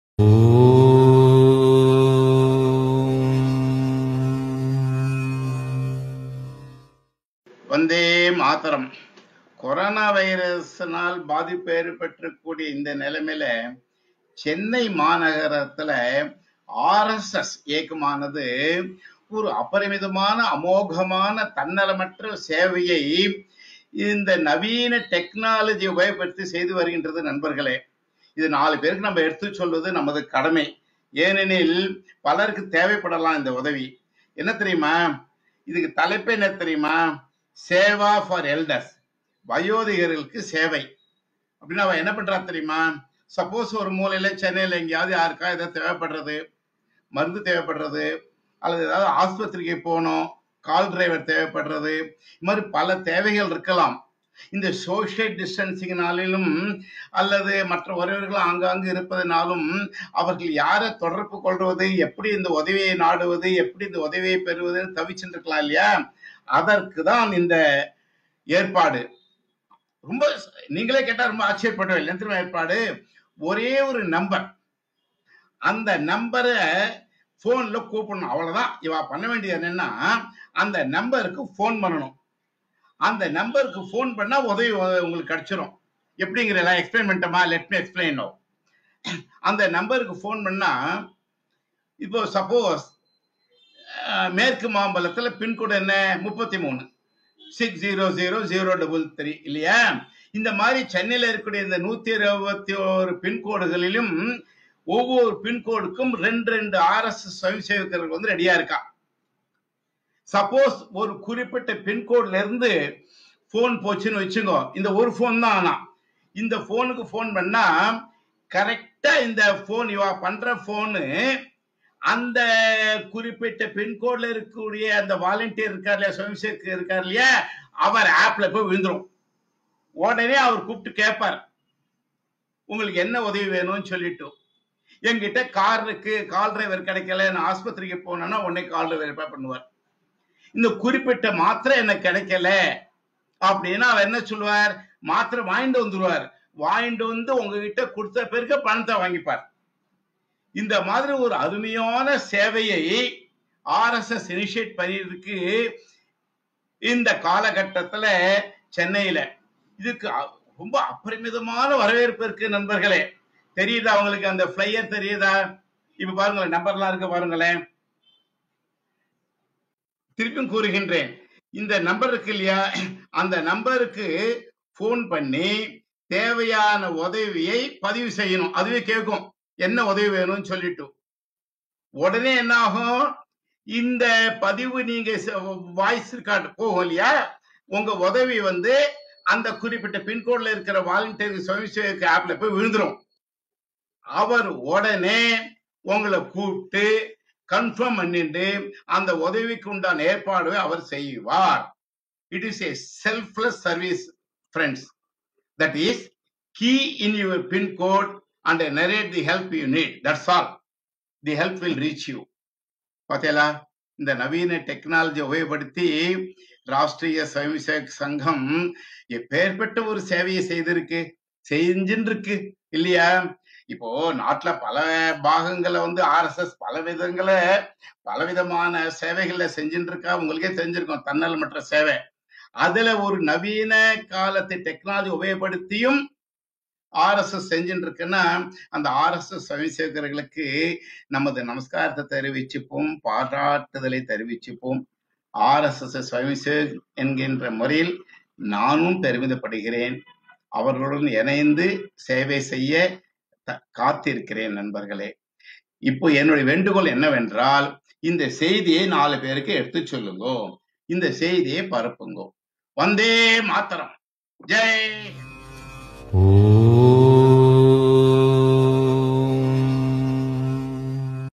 ஆர்எஸ்எஸ் (RSS) ஹெல்ப் லைன் - சென்னையில்: கொராணாவைரஸ் பாதிப்பு மீட்பு பணி. Just amazing.
ஆர் எஸ் எஸ் (RSS) எவ்வளவு வித்யாசமாக யோசித்து திட்டமிட்டு நவீன டெக்னாலஜி மூலம் இந்த கொராணாவைரஸ் பாதிப்பு மீட்பு பணியை சென்னையில் செய்து வருகிறார்கள் பாருங்கள். இதனால் பயனடைந்து வருபவர்கள் ஏராளம், ஏராளம். குறிப்பாக வயதானவர்கள் பயனடைந்து வருகிறார்கள்.
It is Pincode based registration. Of course even without pincode details also, one can register. There is a provision for this also . Here is the procedure. If a call is registered without Pincode, the call will be received by the central team. The members in the Central Team will speak to the called person and get details of area and identify the pincode and assign this call to the particular swayamsevek in that particular pincode and that swayamsevak will start addressing the call